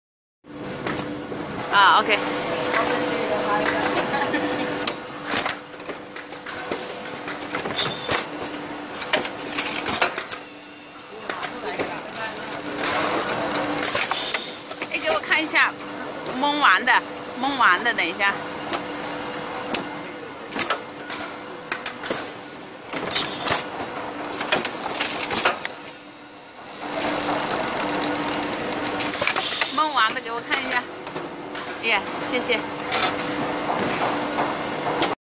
YUTAI Electro-hydraulic system Toe Lasting Machine#
The machine is equipped with required tools (pincers, wipers) for lasting different kinds of shoes (sports, safety boots…) with the ability to change the tools easily.